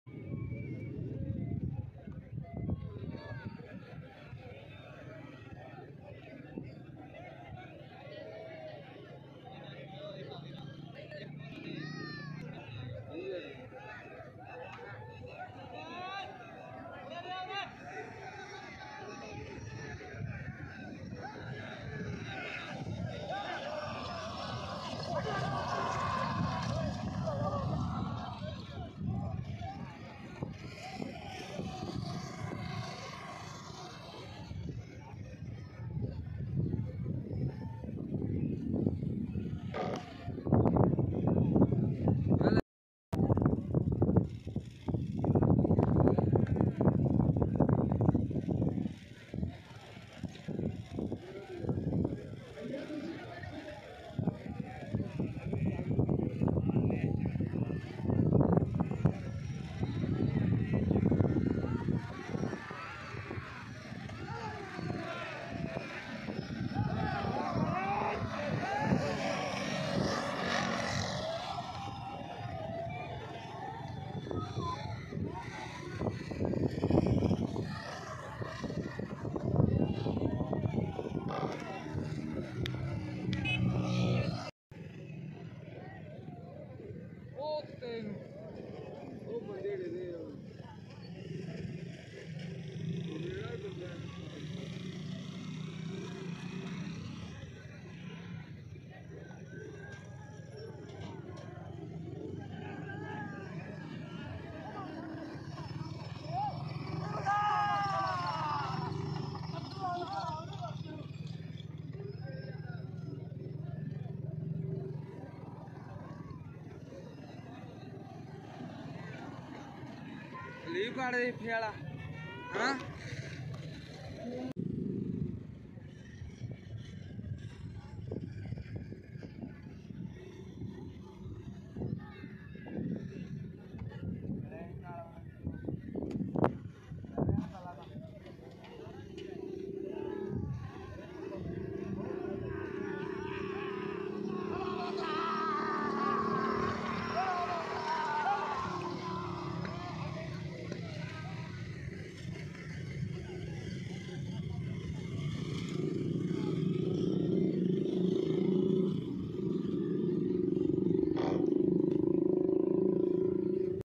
ڈرون کیمرے سے۔4.6.2023.آل پاکستان اوپن بیل گاڑی ریس گراؤنڈ پیر محل4K رزلٹ مین مکمل ریس
مزید اس طرہ کی بیل گاڑی ریس کی اچھی اچھی ویڈیو دیکھنے کیلئے ہمارے چینل کو سبسکرائب کرین شکریہ
ڈرون کیمرے کی ویڈیو 4k رزلٹ مین